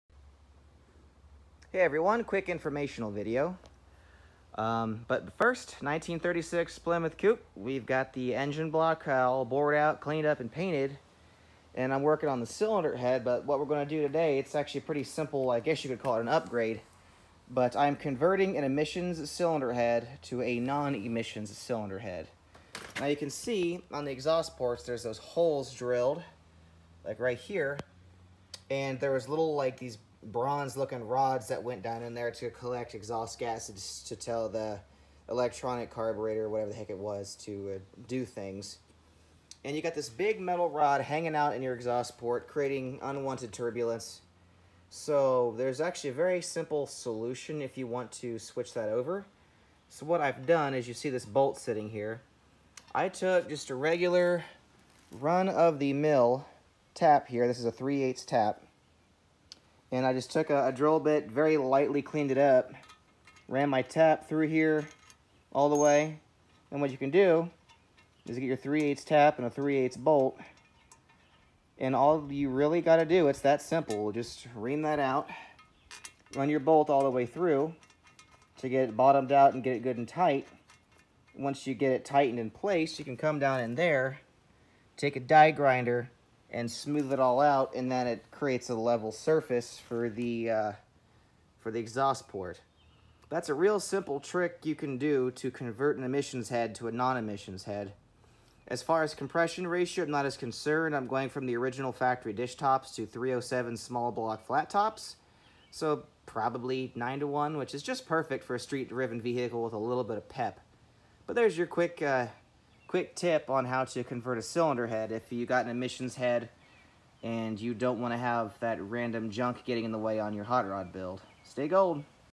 Converting an emissions cylinder head to non emissions: Chevy 250
Real quick video on making your malaise era head more useful.